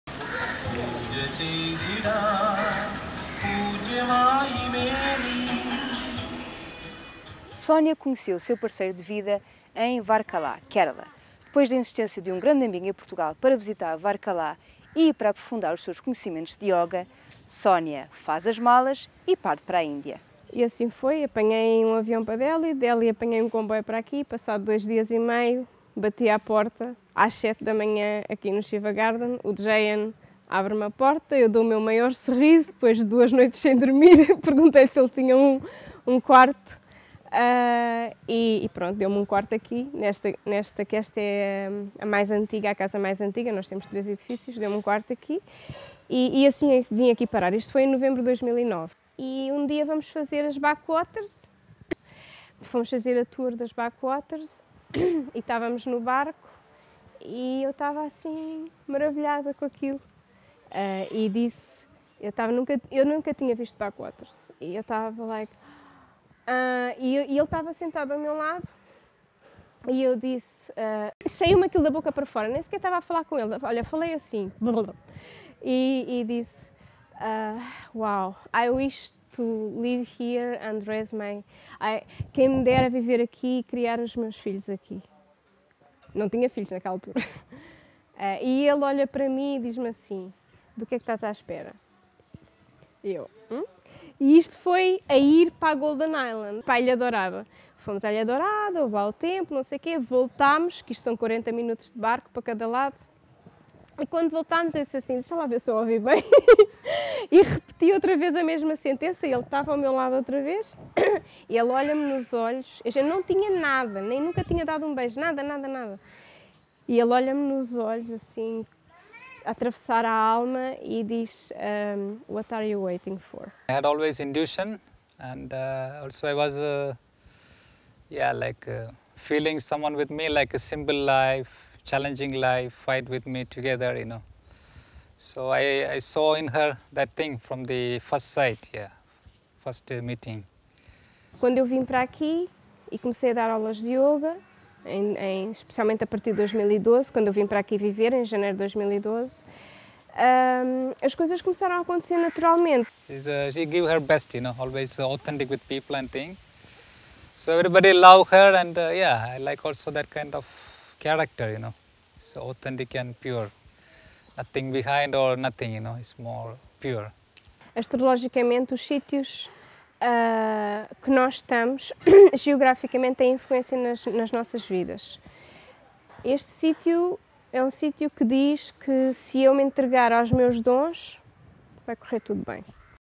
Portuguese in India - Sonia Indigo - Hora dos Portugueses - Ep 53
Sonia Indigo fell in love and decided to move to Kerala. She teaches reiki, yoga, numerology, astrology etc. She has many skills. She is kind, open hearted person and it's impossible not to like her.
Get to know her more in this episode.